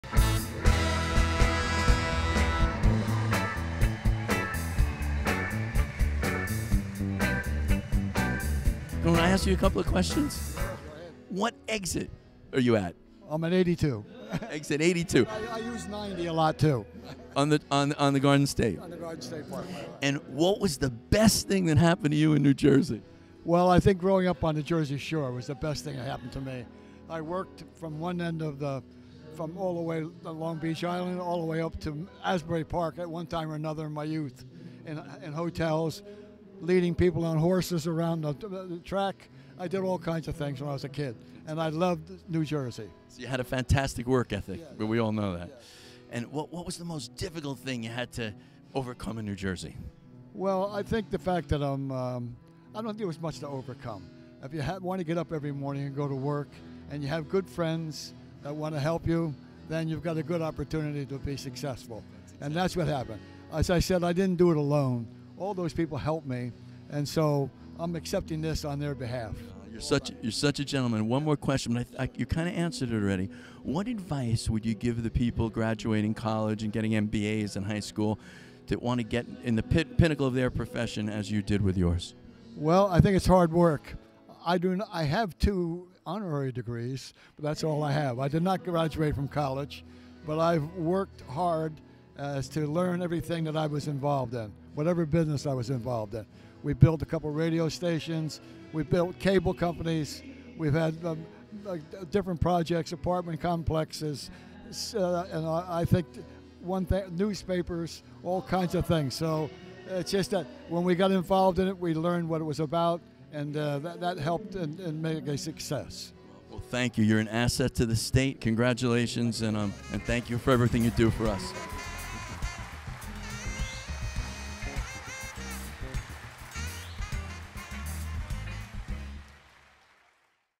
Backstage with Joseph Buckelew - NJ Hall of Fame 2018
Al Sapienza, from "The Sopranos" & "House of Cards", interviews Class of 2017 inductee Joseph Buckelew backstage at the NJ Hall of Fame induction ceremony.